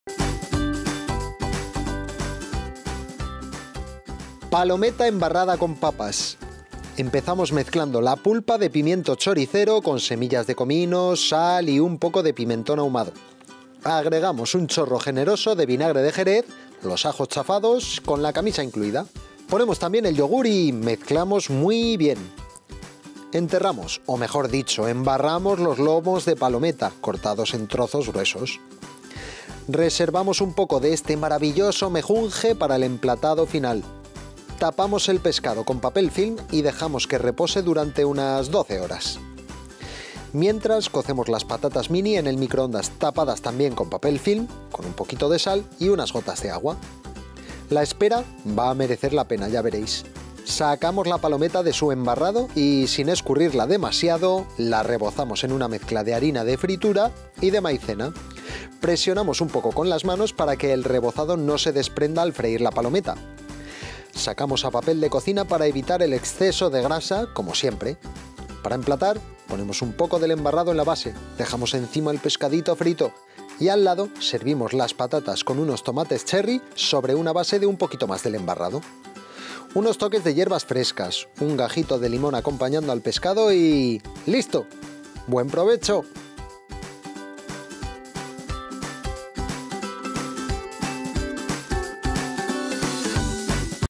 PALOMETA EMBARRADA CON PAPAS - LPL PGM 1979 RECETA 02
Ingredientes para 4 personas:
2 lomos de palometa.
Para el embarrado, 130 g de pulpa de pimiento choricero, 2 dientes de ajo, 1 chorrito de vinagre de Jerez, orégano, pimentón ahumado, semillas de comino y2 cucharadas de yogur griego.
Además, 2 partes de harina fritura,1 parte de maicena, tomates cherry, aceite de oliva suave, sal, pimienta negra y aceite de oliva virgen extra.

Elaboración:
Empezamos mezclando la pulpa de pimiento choricero con las semillas de comino, sal y un poco de pimentón ahumado. Añadimos un buen chorro de vinagre y los ajos chafados, camisa incluida. Ponemos también el yogur y mezclamos muy bien. Enterramos, mejor dicho, embarramos los lomos de palometa, cortados en trozos gruesos. Reservamos un poco de este mejunje maravilloso para el emplatado final. Tapamos el pescado con papel film y dejamos que repose durante unas doce horas.
Mientras, cocemos las patatas mini en el microondas, tapadas con papel film, como siempre.
La espera va a merecer la pena, ya veréis. Sacamos la palometa de su embarrado y, sin escurrirla demasiado, la rebozamos en una mezcla de harina de fritura y maicena. Presionamos un poco el rebozado antes de meter la palometa a freír en aceite bien caliente. Sacamos a papel de cocina para evitar el exceso de grasa.
Para emplatar, ponemos un poco del embarrado en la base y dejamos encima el pescado frito. Al lado, servimos las patatas, unos tomates cherry y un gajo de limón. Unos toques de hierbas frescas y… palometa embarrada lista. Que os aproveche.